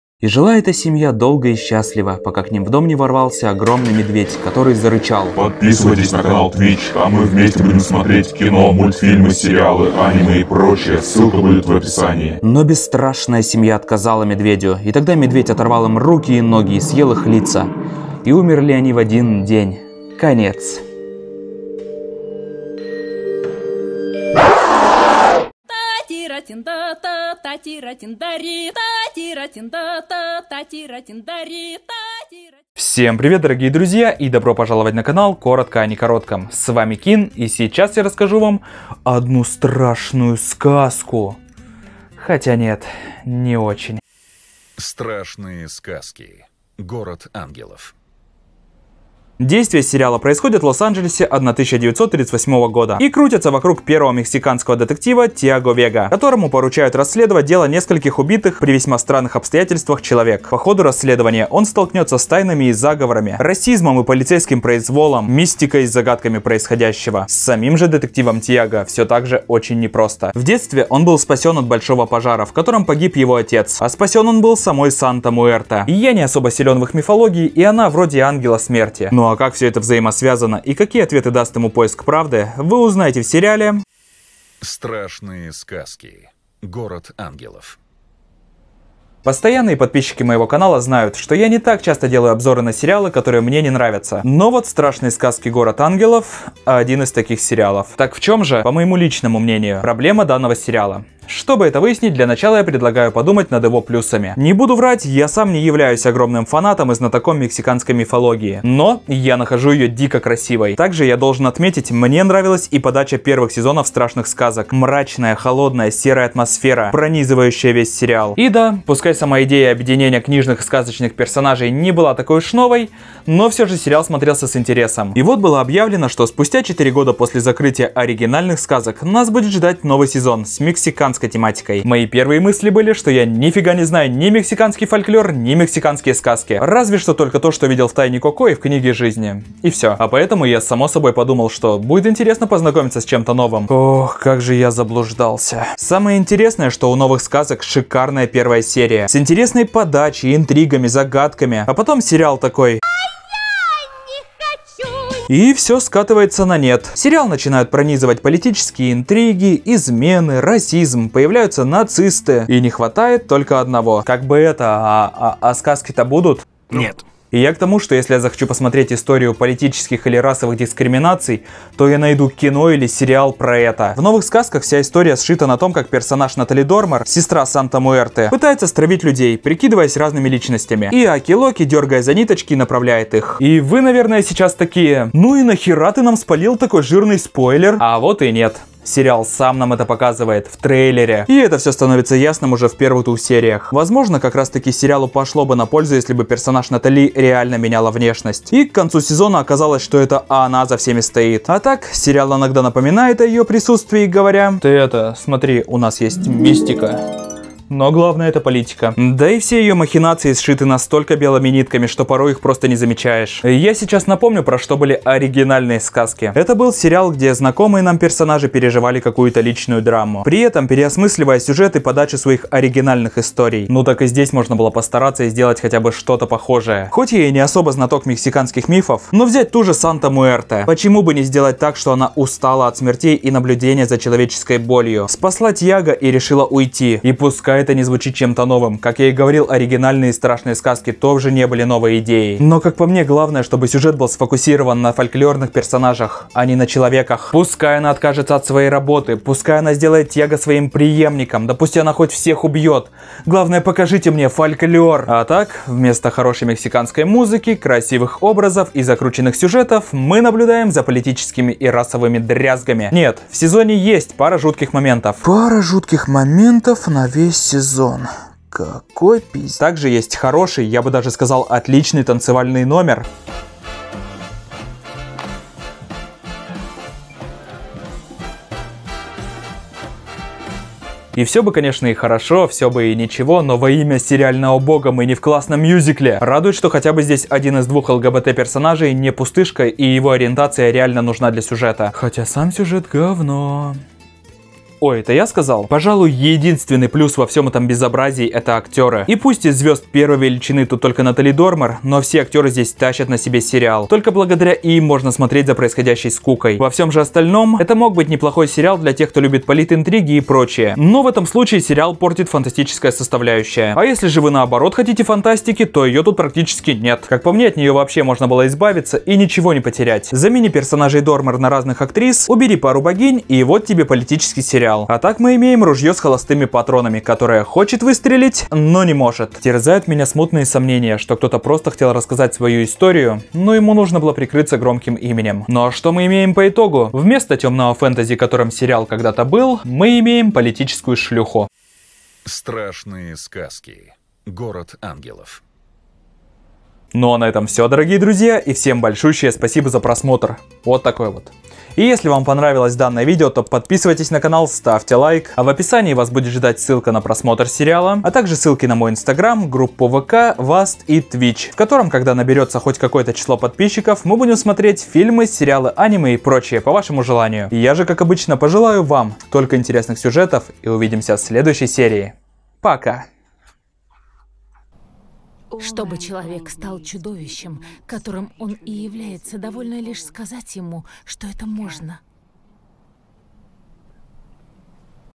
📽Вкратце про (НЕ) СТРАШНЫЕ СКАЗКИ : ГОРОД АНГЕЛОВ / PENNY DREADFUL 💀🔥 [Обзор Сериала ShowTime]
Помочь каналу ❤

Сбербанк-Visa Classic- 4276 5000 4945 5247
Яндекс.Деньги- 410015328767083

Страшные сказки: Город ангелов / Penny Dreadful: City of Angels

Сухие факты:
Год выхода- 2020
Продолжительность серии- 45
Количество сезонов (2020 год)-1
Количество серий-10
Рейтинг IMDb- 6.00
Студия- ShowTime
В главных ролях- Натали Дормер, Дэниэл Дзоватто

Слоган- "The tale is just beginning"

Смотрел в озвучке- Amedia

Смотрел на сайте (!Внимание! Сайт не проверен антивирусом и в сериях присутствует реклама)